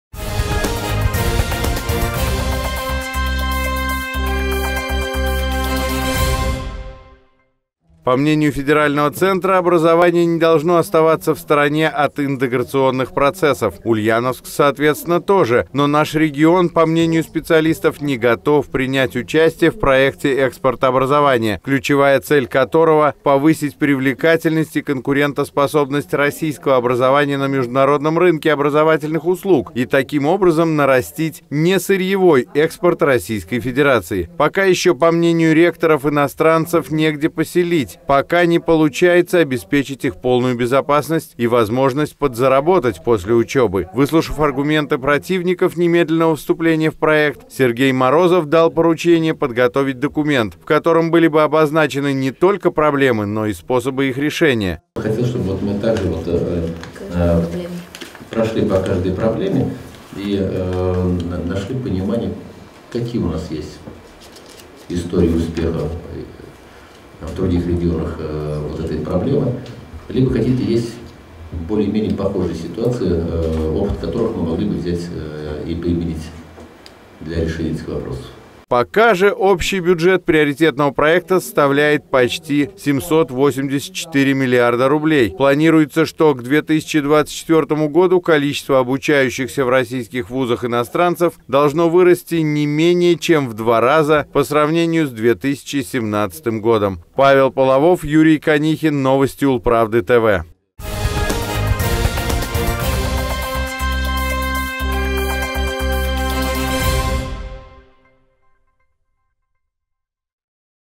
Ульяновская область готовится к реализации федерального проекта
Может ли Ульяновская область вступить в федеральный проект «Экспорт образования»? Этот вопрос министры и ректоры ульяновских ВУЗов обсудили с губернатором Сергеем Морозовым.